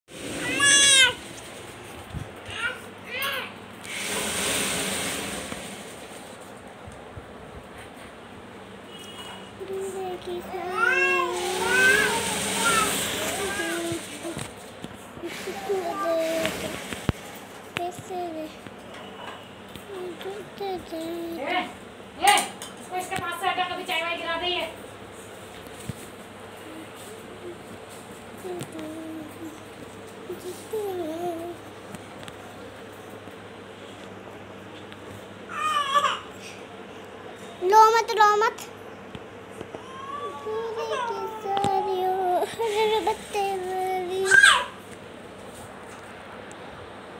Bushra's video edited by her elder sis Afsheen who is only 6 years old
Video from Zaheer